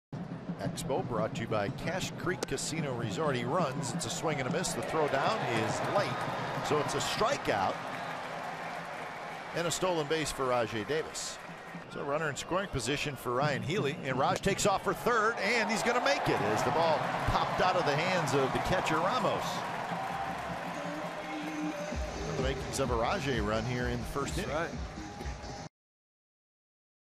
TB@OAK: R. Davis steals second and third in the 1st
Rajai Davis steals second and third base in the bottom of the 1st inning, notching his 16th and 17th stolen base of the season